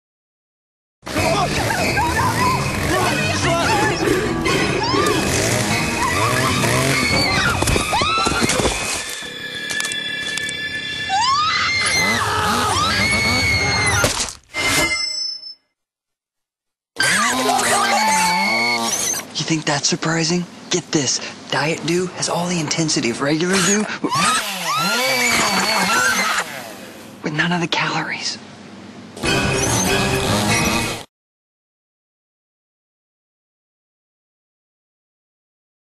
Ferret Chainsaw Commercial - Uncut
Diet Mountain Dew Commerical...featuring a ferret with a chainsaw. You can't get much better than that.

Commerical (c) PepsiCo Diet Mountain Dew Commerical...featuring a ferret with a chainsaw. You can't get much better than that.